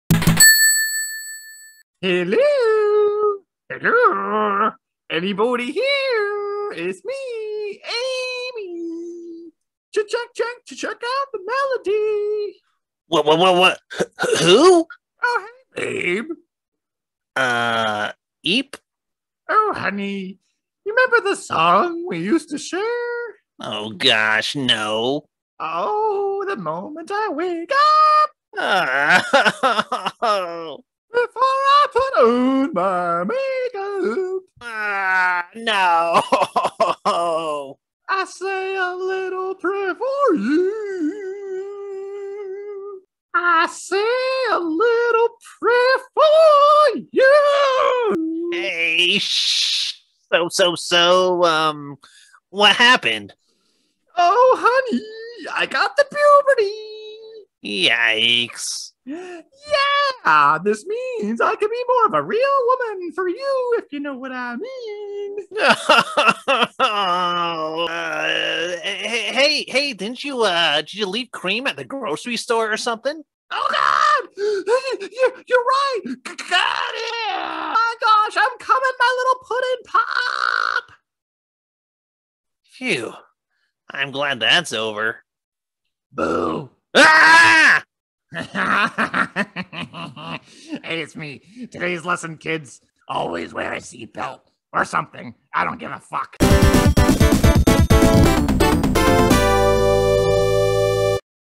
Shadow-Cast Ep 13 AMY IS HERE!
A familiar pink hedgehog girl goes thru puberty...